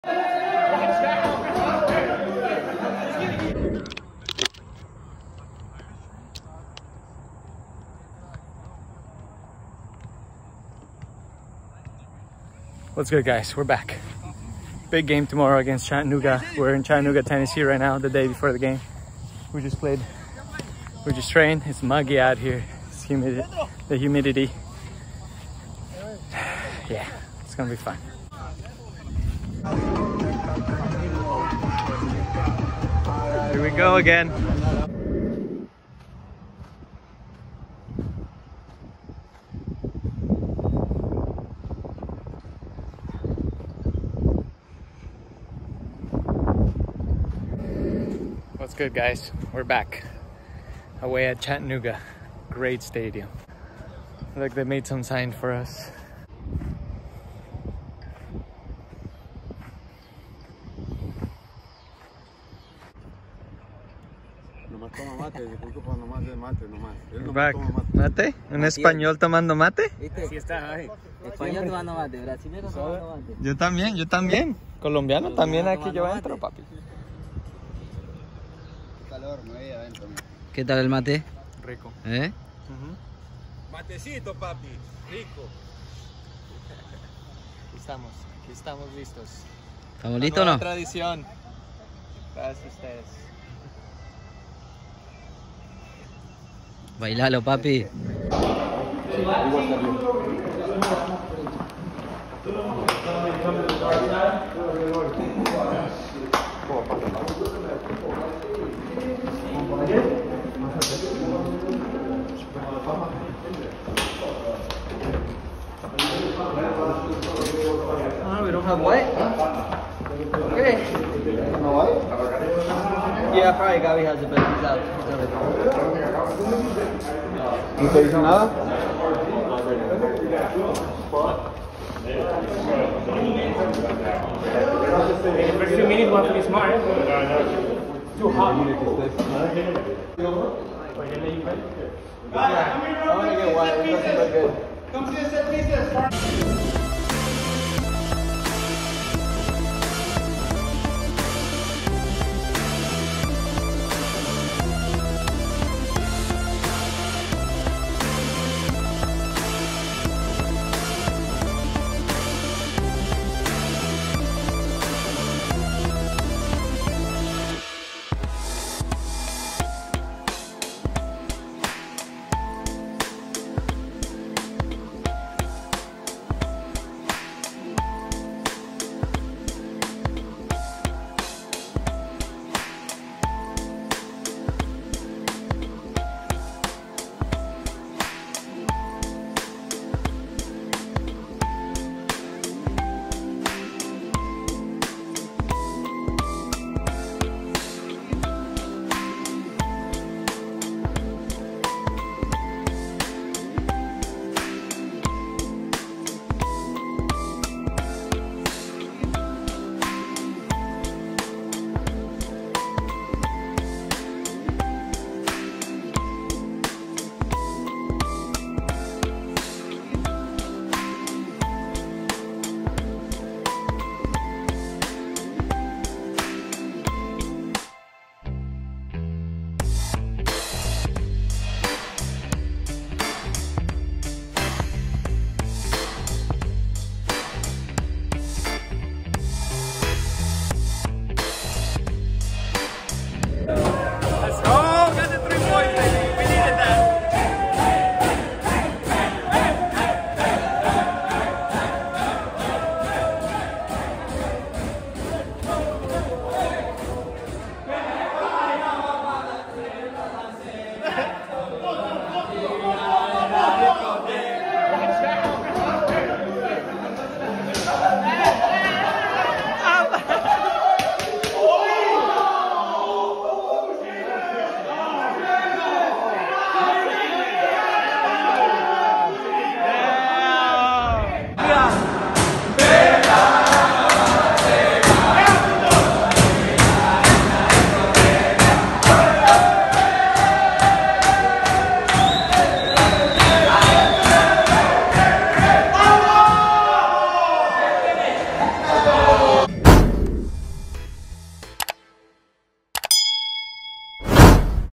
LA Force @ Chattanooga FC Vlog | Nisa Fall Season
Hey guys, this is a video is a behind the scene look of our away game at Chattanooga and a highlight of my touches. Need a win badly after drawing 3 in a row.

Follow the season on my instagram! Cheers.